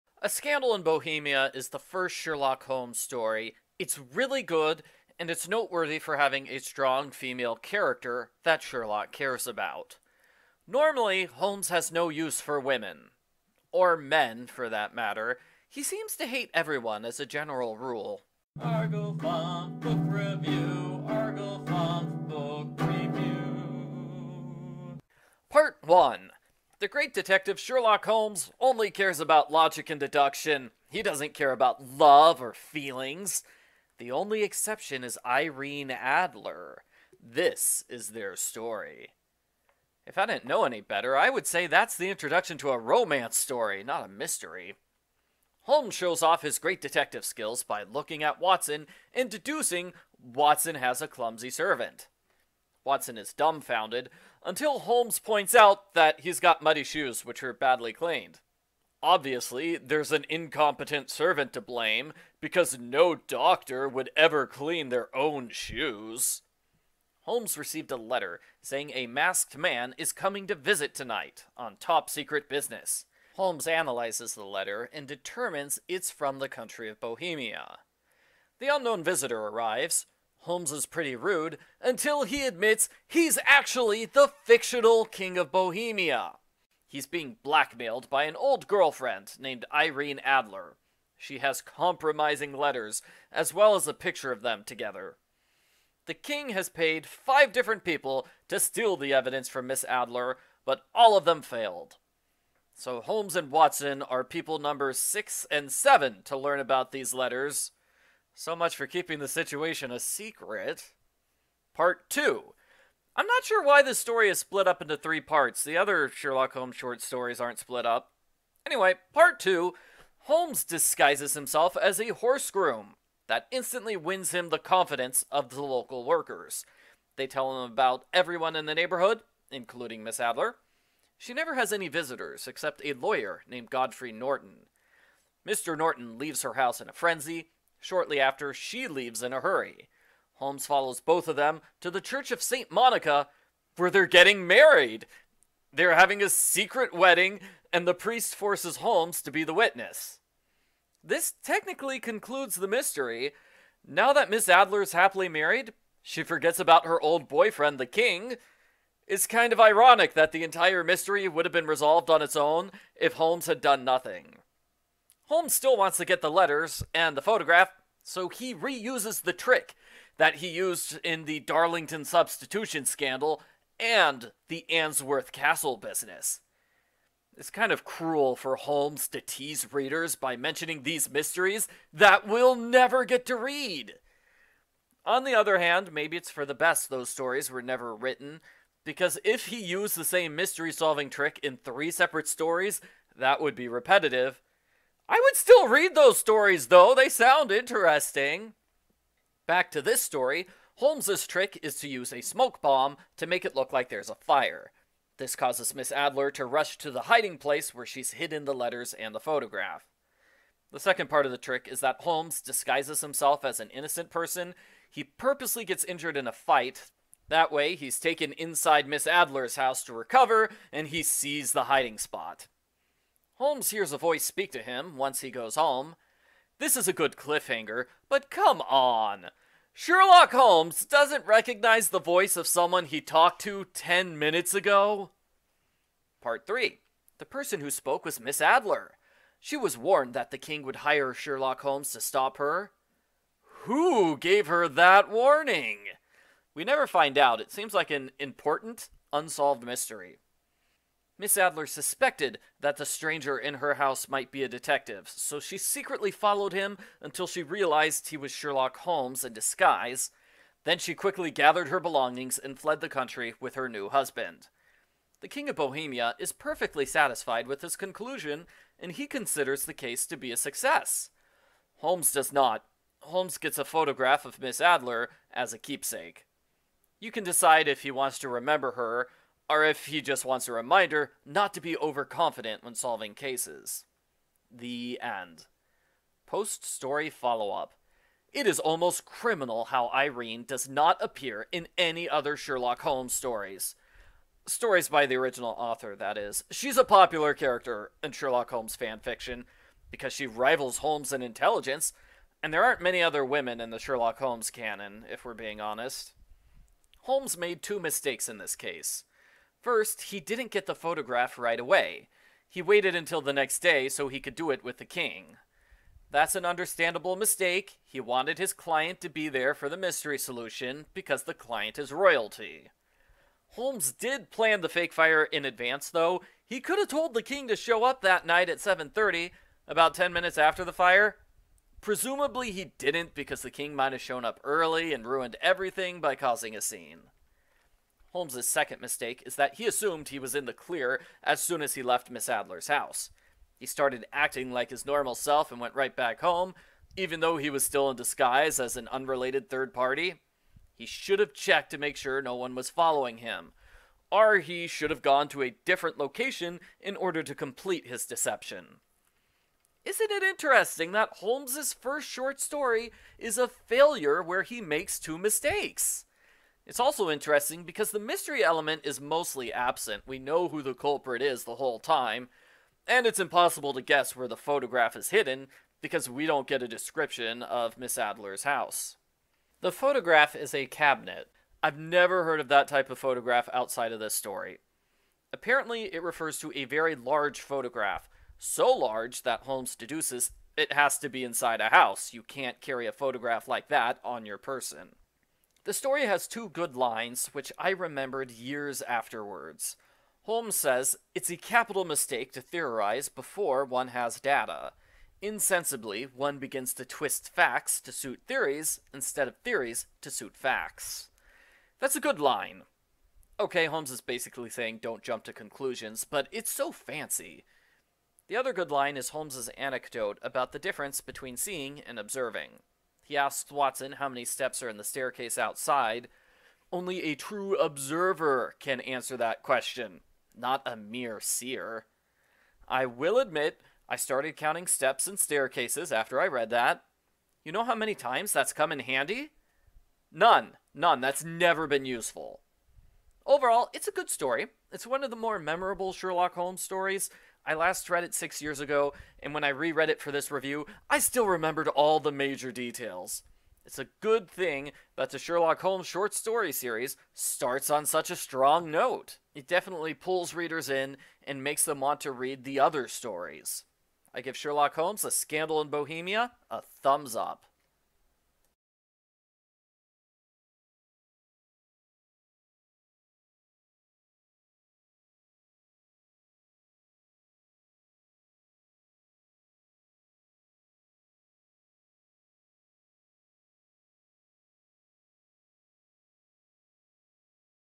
Sherlock Holmes: A Scandal in Bohemia - Review
A review of the first Sherlock Holmes short story, in which the King of Bohemia hires Holmes to get compromising letters from his former girlfriend. The woman in question is Irene Adler, who manages to outsmart Holmes.

I give this story a thumbs up.